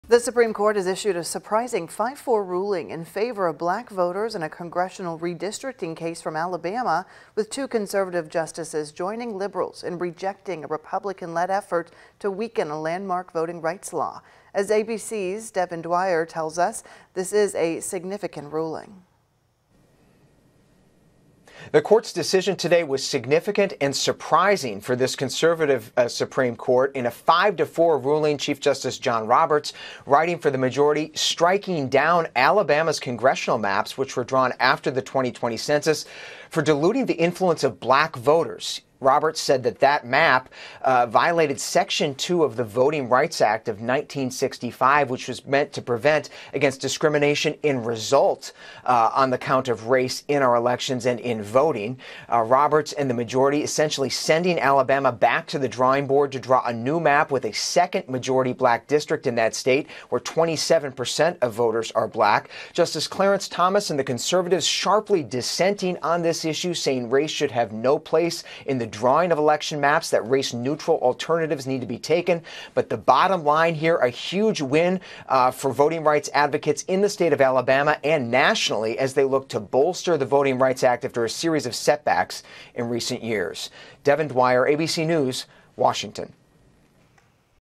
Supreme Court strikes down Alabama's congressional maps
The congressional maps were drawn after the 2020 census.